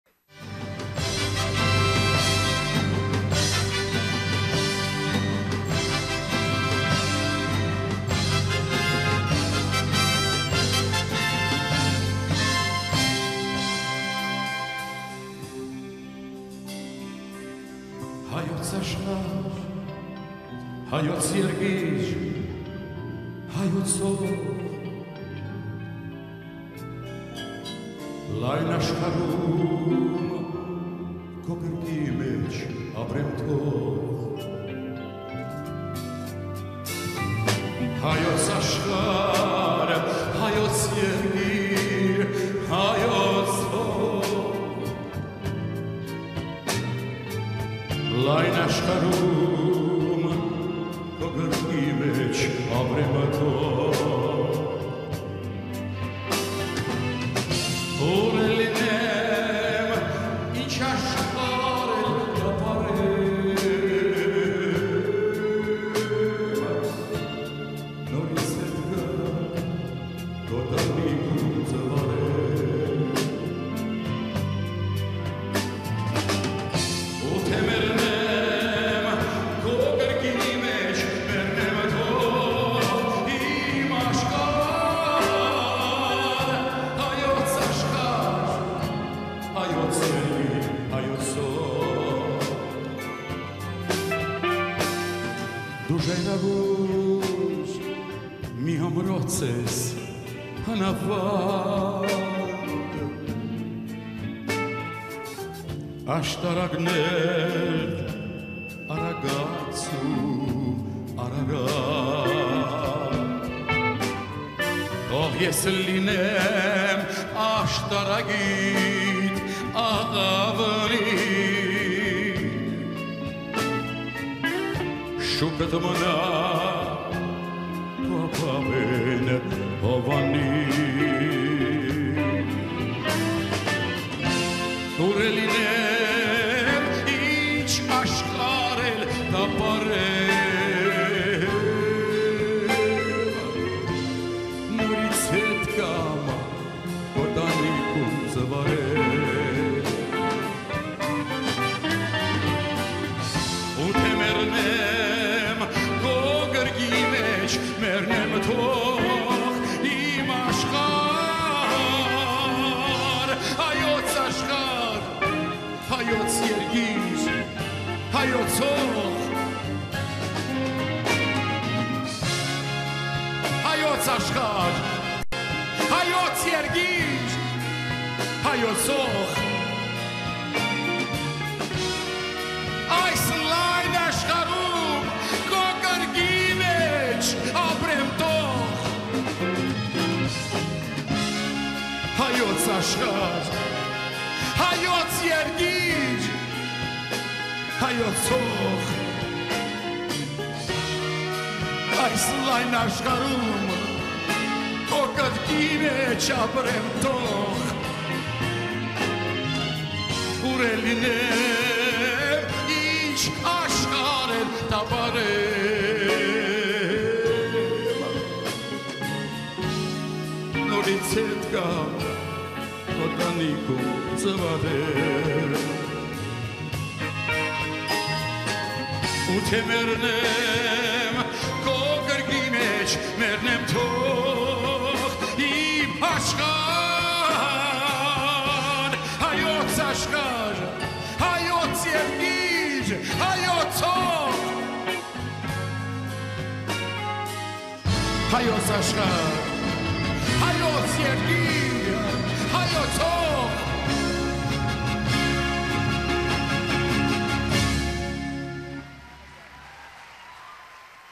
Georgie Minassian performs at the very first Armenian Music Awards 1998
Georgie Minassian performs in the very first Armenian Music Awards October 7 1998 held at the legendary Alex Theatre in Glendale California and hosted by Manuel. Produced by Peter Bahlawanian